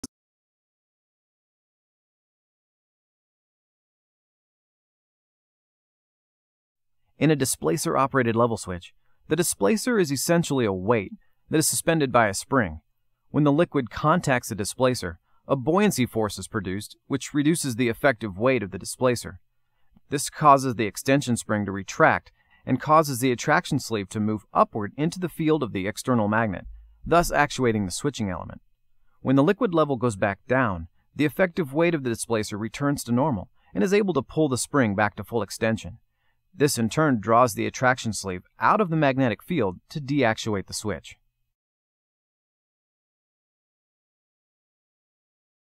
Displacer Operated Level Switch Operation
Operating principle of SOR displacer operated level switch in liquid level applications.